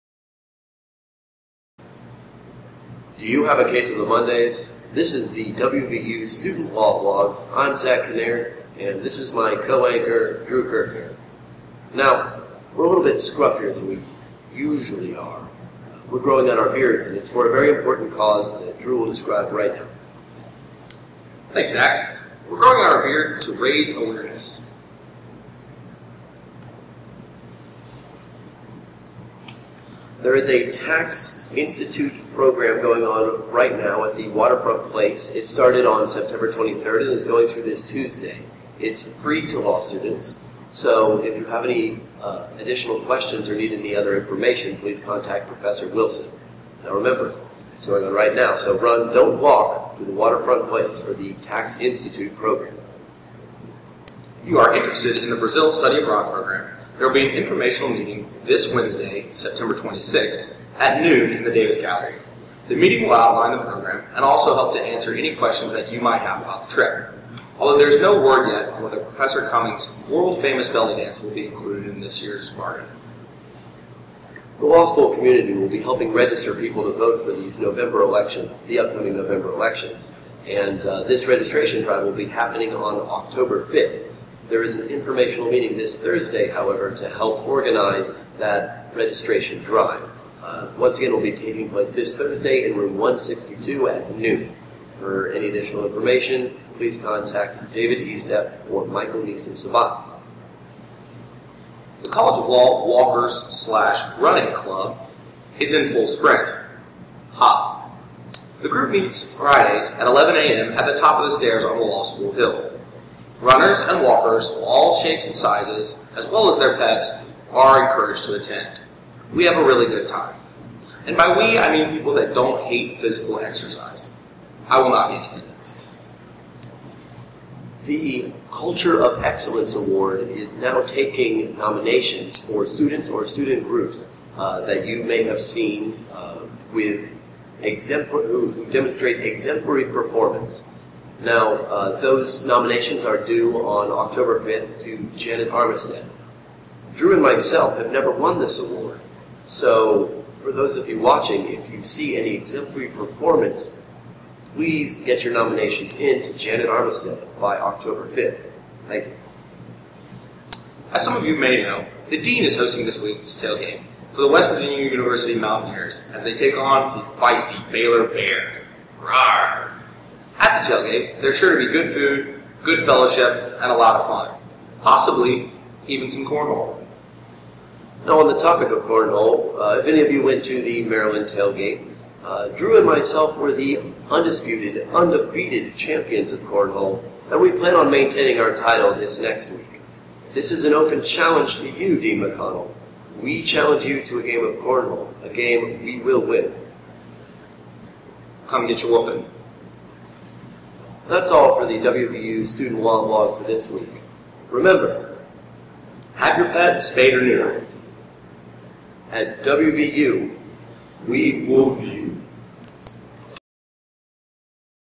WVU Student Law Vlog - Week 6, 2012
A few events coming up this week, the Culture of Excellence Award, and a challenge to the Dean.